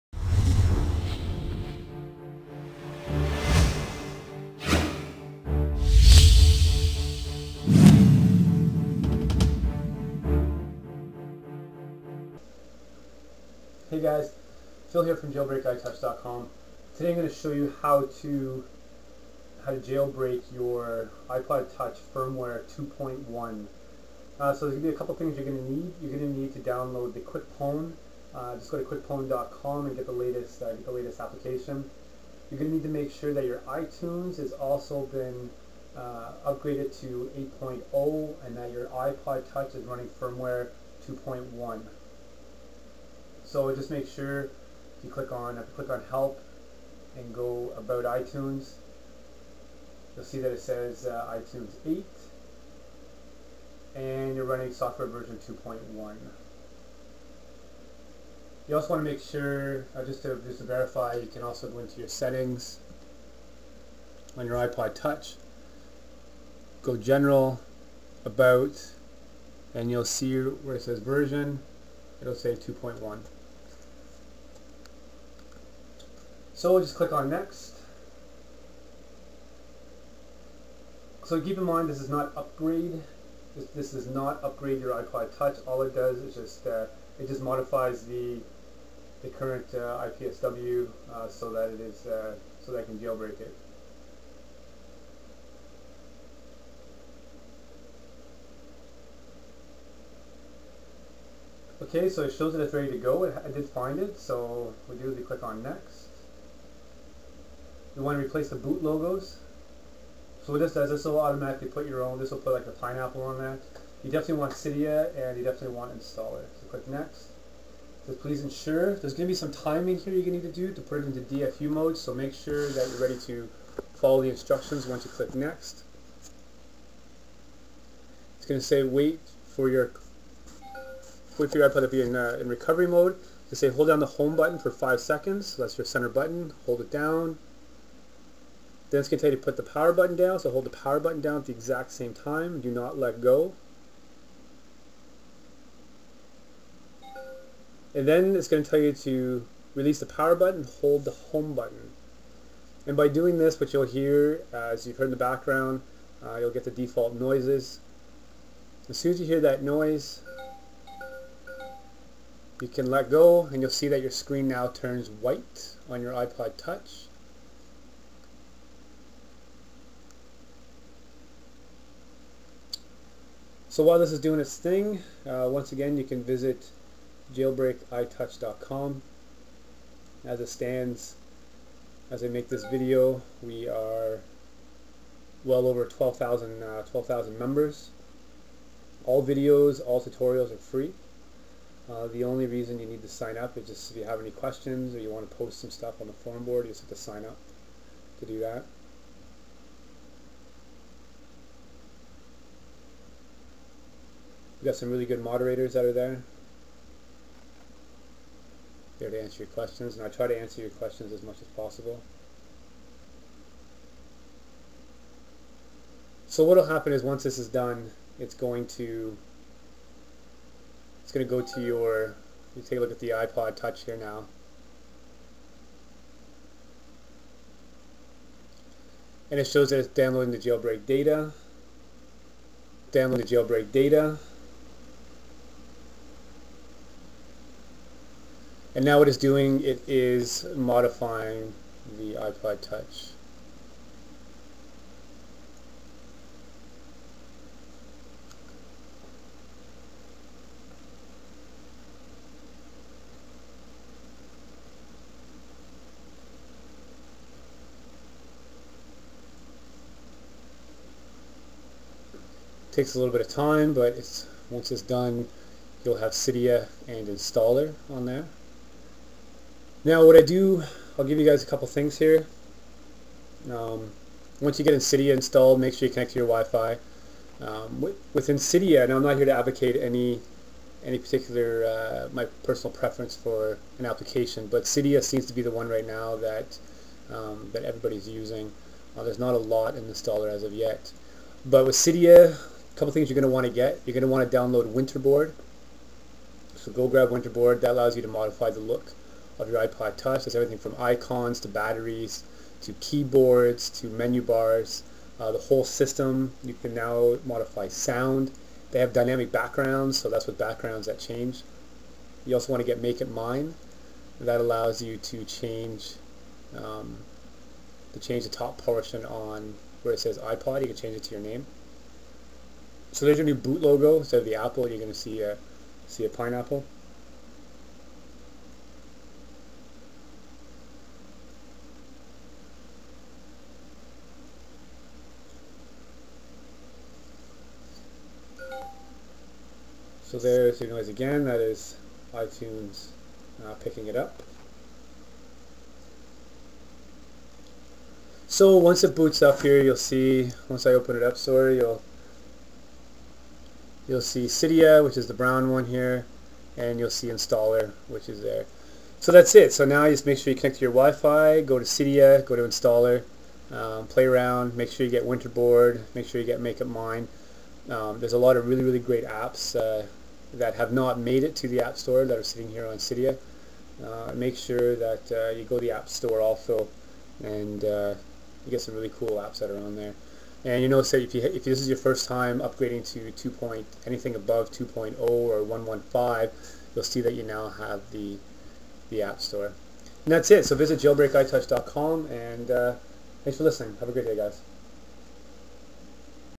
Jailbreak iPod Touch Firmware 2.1 using QuickPwn
this is for 1st Generation - 2nd (one with speakers have not been jailbroken)

Jailbreak iPod Touch Frirmware 2.1 using QuickPwn

you need to make sure that you are already at 2.1 before you can jailbreak it.  It will be located here on your PC
C:\Documents and Settings\Phil(replace)\Application Data\Apple Computer\iTunes\iPod Software Updates\iPod1,1_2.1_5F137_Restore.ipsw